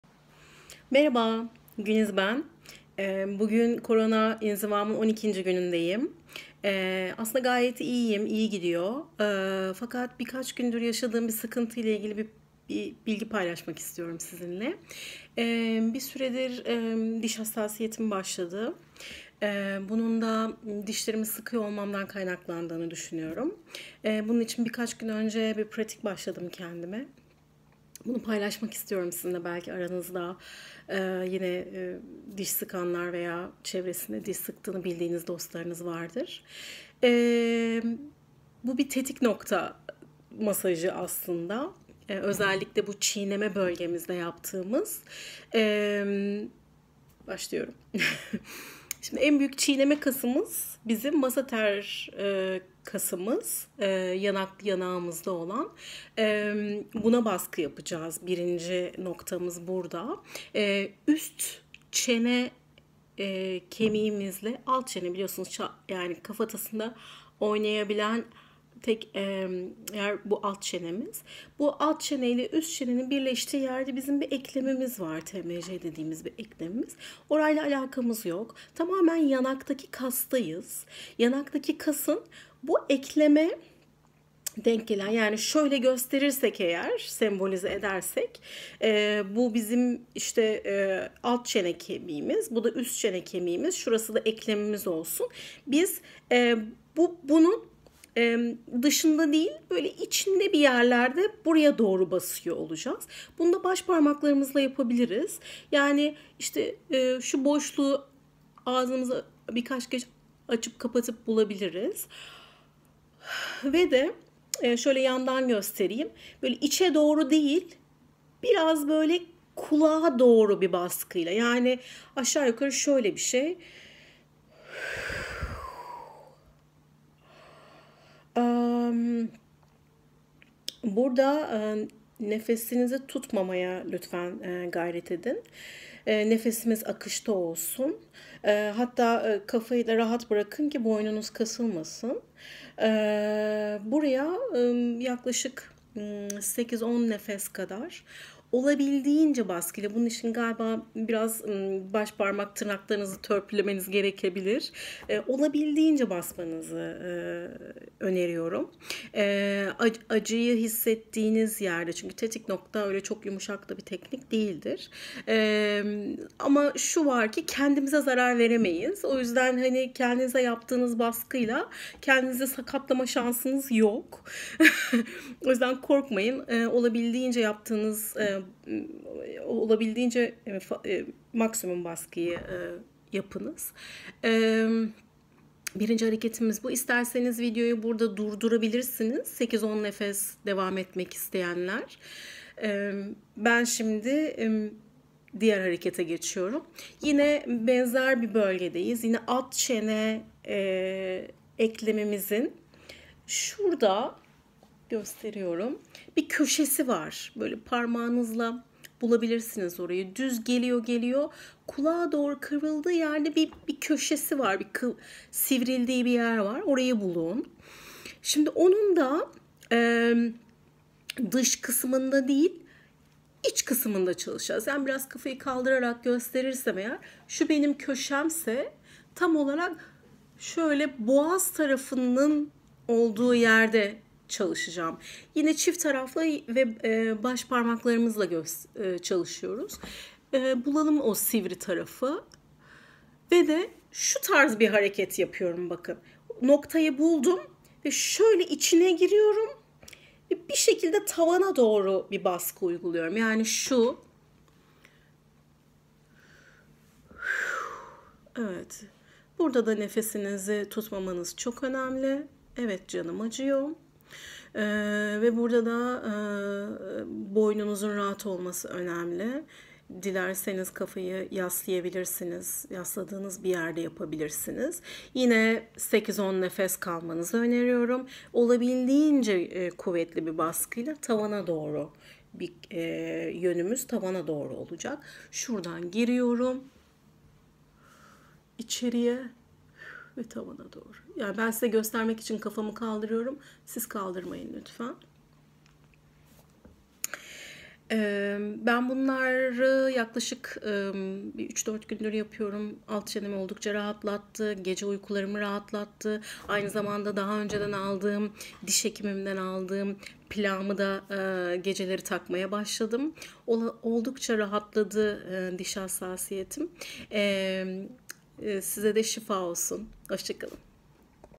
Diş Sıkma Problemi için Kendi Kendine Tetik Nokta Masajı
Diş sıkma problemim için kendi kendime yapabildiğim bir tetik nokta masaj pratiğimi paylaşmak istedim. Umarım ihtiyacı olan herkese faydası olur🙏